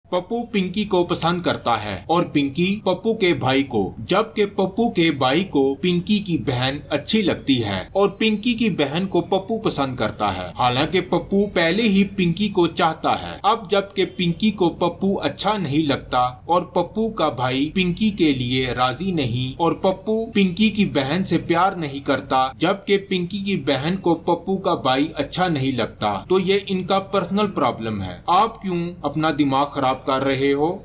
Pappu Pinko ko | Funny Jokes | Funny Jokes in Urdu
Pappu Pinko ko pasand karta hay, aur Pinki Pappu k bhai ko…
Jab k Pappu k bhai ko Pinki ki behan achi lagti hai aur Pinki ki behan ko Pappu pasand karti hai. Halaki Pappu pehle hi Pinki ko chahta hai….
Ab jab-k Pinko ko Pappu acha nahin lagta aur Pappu ka bhai Pinki k liye razi nahi hay aur Pappu Pinki ki behan se pyar nahin karta jab k Pinki ki behan ko Pappu ka bhai acha nahin lagta…

To ye unka personal problem hay. Aap kyon apna dimag kharab ker rahe ho